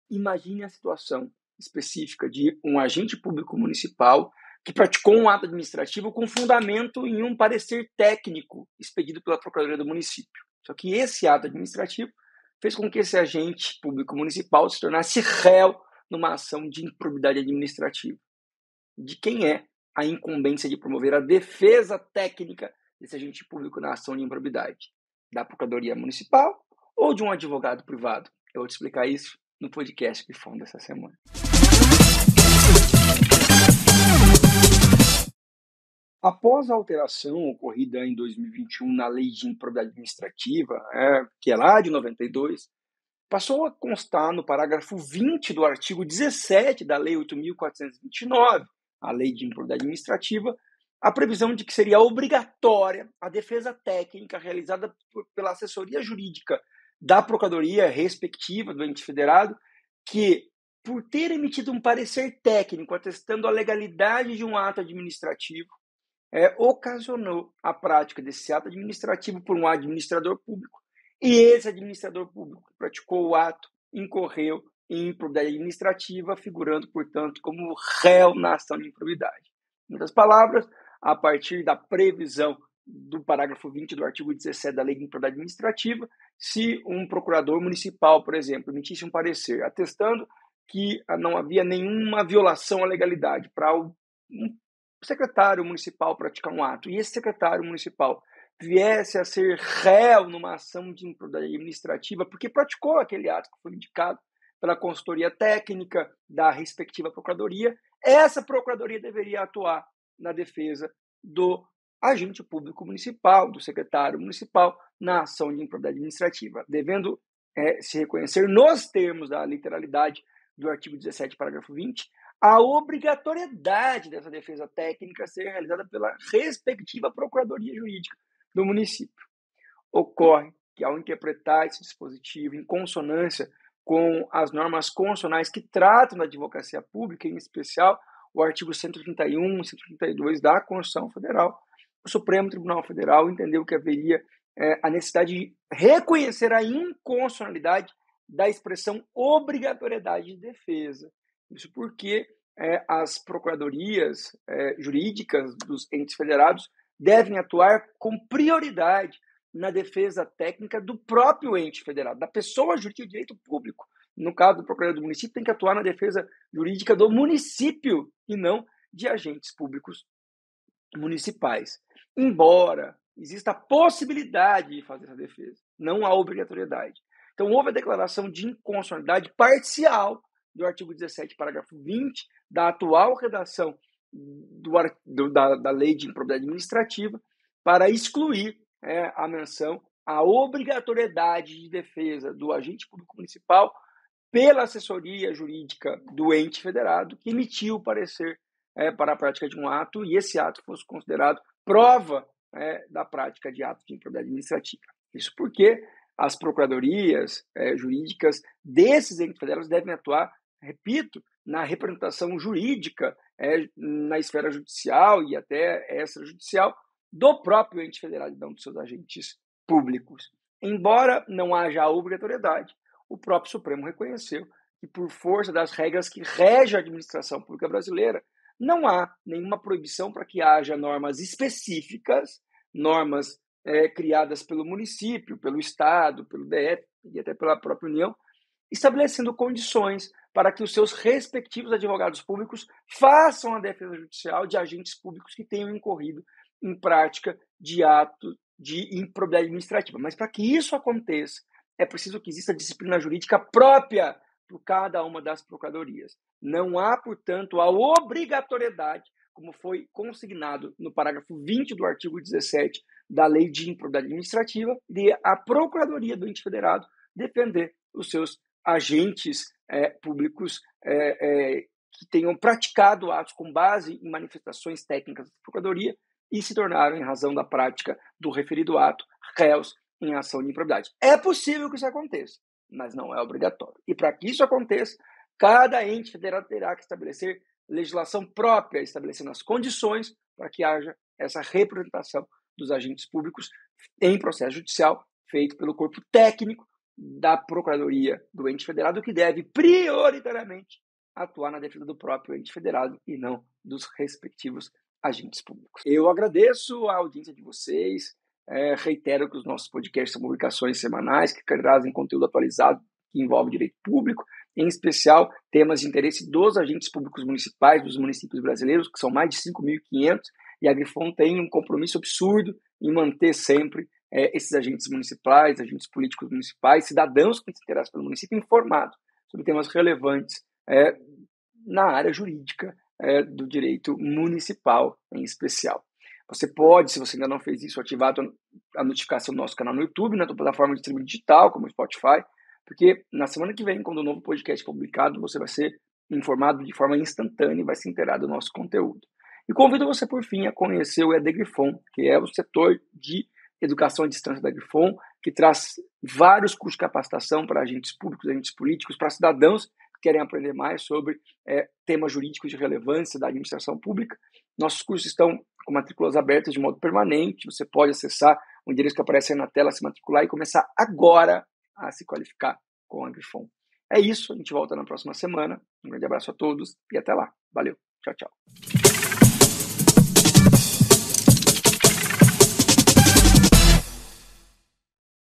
A advocacia pública é obrigada a defender servidores em ação de improbidade administrativa?
O Podcast Grifon #268 analisa a decisão do STF que fixou a orientação de que não existe obrigatoriedade de defesa judicial realizada pela Advocacia Pública em prol de agente público, que figura como réu em ação de improbidade administrativa em decorrência da prática de ato administrativo fundado em parecer técnico exarado pela respectiva Procuradoria (ADI nº 7042/DF).

Também disponível nas plataformas:
Spotify, Pocket Cast e Castbox.